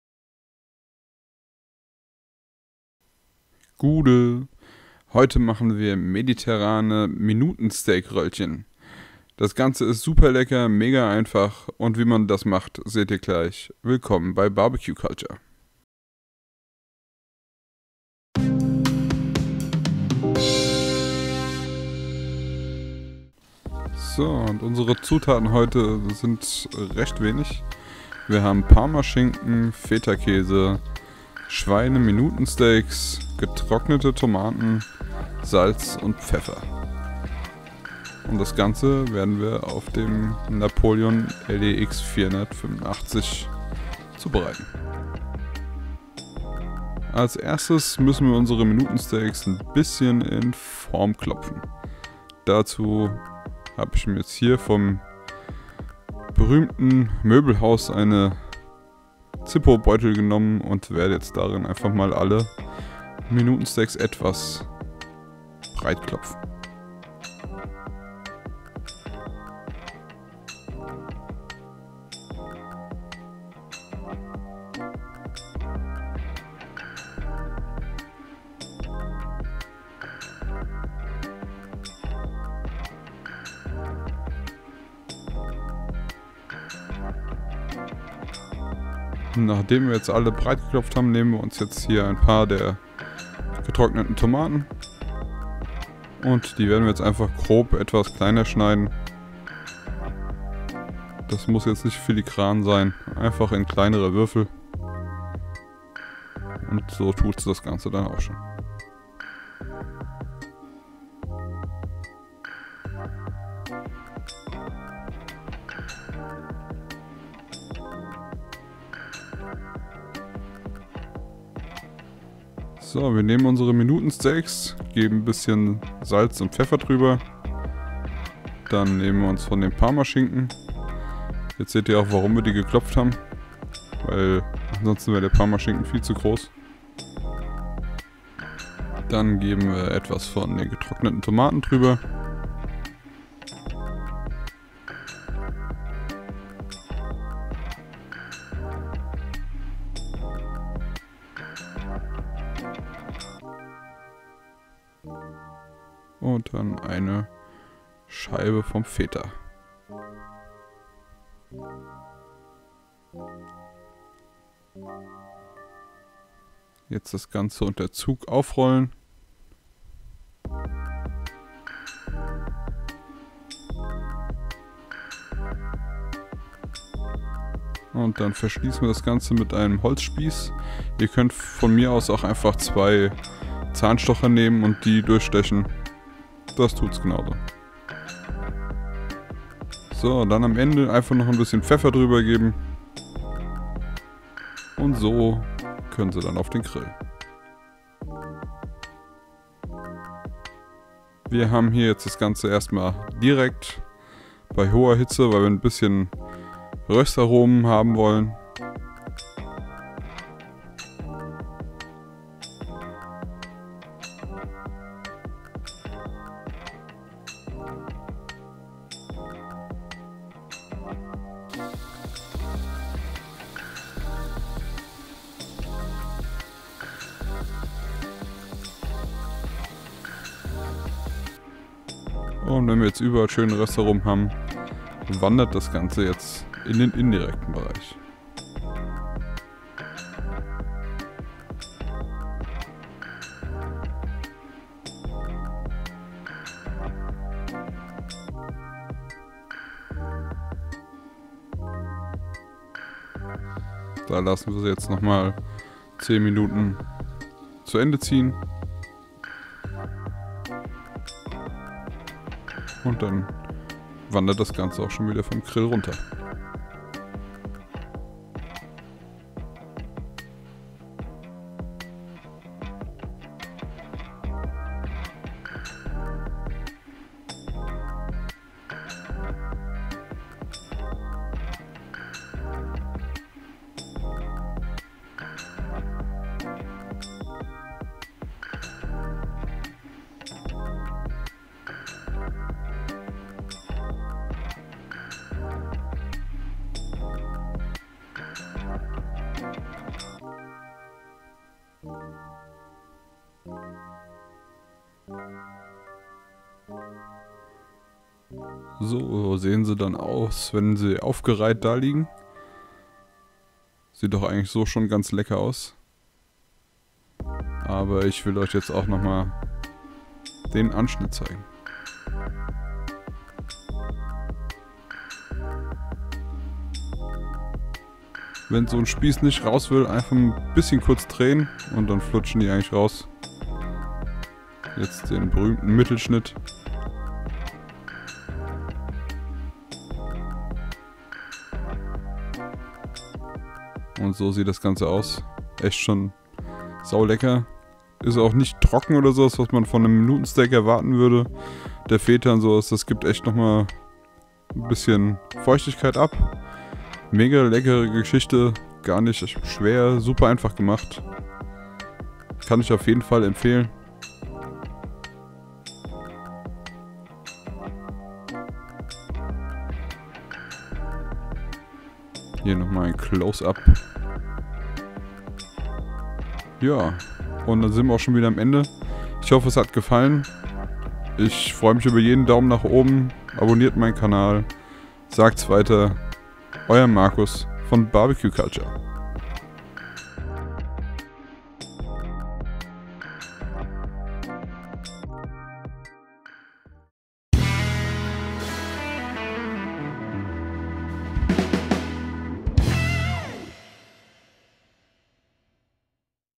Mediterrane Minutensteak Röllchen - sehr leckere Geschichte
Dieses Mal machen wir mediterrane Minutensteak Röllchen vom Grill.
Einfach, lecker und schnell gemacht.

Zutaten:
5 Minutensteaks
5 scheiben Parmaschinken
getrocknete Tomaten
Fetakäse
Salz
Pfeffer
Holzspieße

Zubereitung:
Zuerst müssen wir die Minutensteaks etwas in Form klopfen so dass wir etwas mehr Fläche gewinnen. Wenn das geschehen ist schneiden wir uns die getrockneten Tomaten in etwas kleinere Stücken so dass wir sie dann gut auf den Minutensteaks verteilen können.

Jetzt legen wir uns eines der Minutensteaks flach aus und geben etwas Pfeffer und Salz darüber. Dann legen wir eine Scheibe des Parmaschinkens darüber.
Darauf kommen dann die getrockneten Tomaten und ein Stück Feta.
Das belegte Minutensteak kann nun mit etwas ziehen aufgewickelt und mit einem der Holzspieße verschlossen werden.
Die Röllchen legen wir jetzt zuerst in den direkten Bereich bei hoher Hitze. Wenn wir von allen Seiten schöne Röstaromen bekommen haben legen wir die Röllchen in den indirekten Bereich um auf die gewünschte KT zu kommen.

Ihr werdet es lieben. Macht es auf jeden Fall mal nach und gebt mir dann mal Feedback wie es war.

Eurer Marcus von BBQ Culture